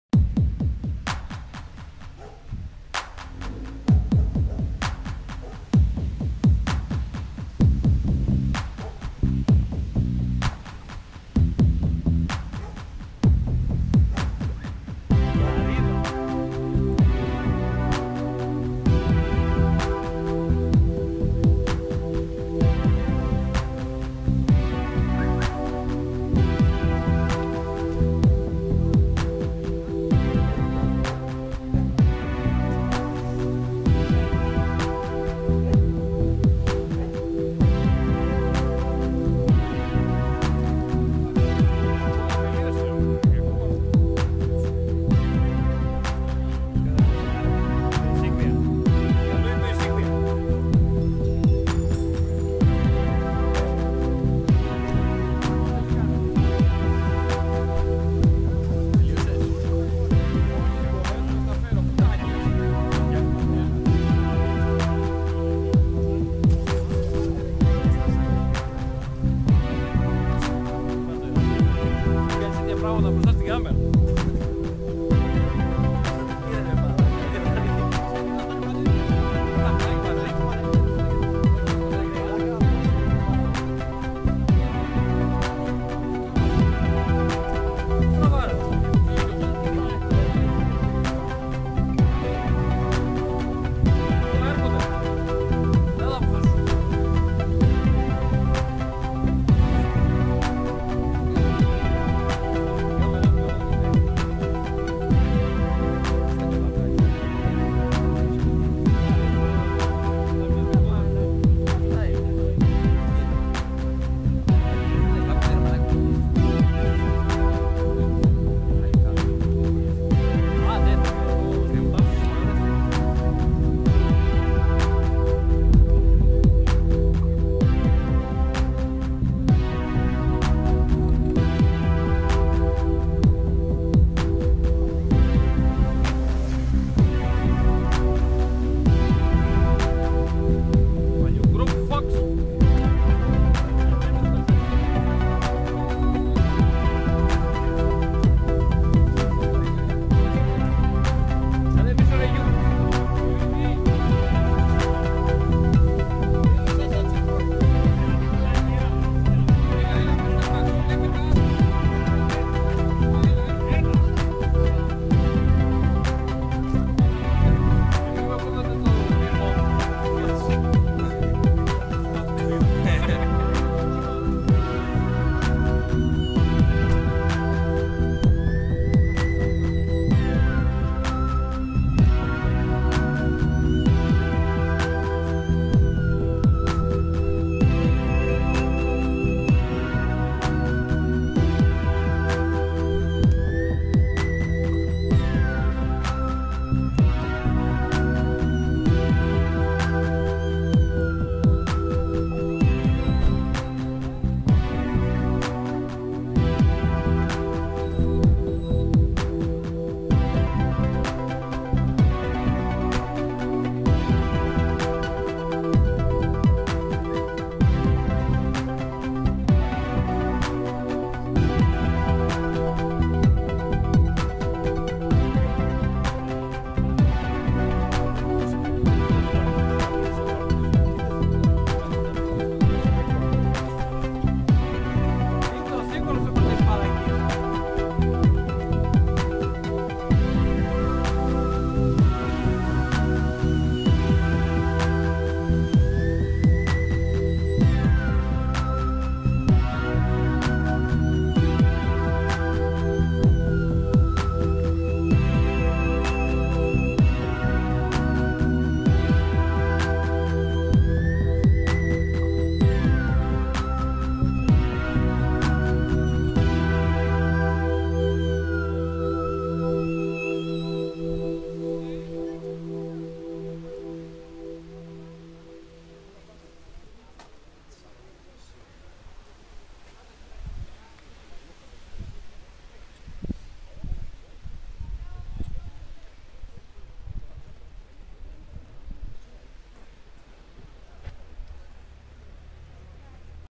Πανελλήνια εκδρομή opelclub.gr στην Βέροια
Απο την Πανελλήνια συγκέντρωση του opelclub.gr στην πόλη της Βέροιας,στο parking που έλαβε μέρος η φωτογράφιση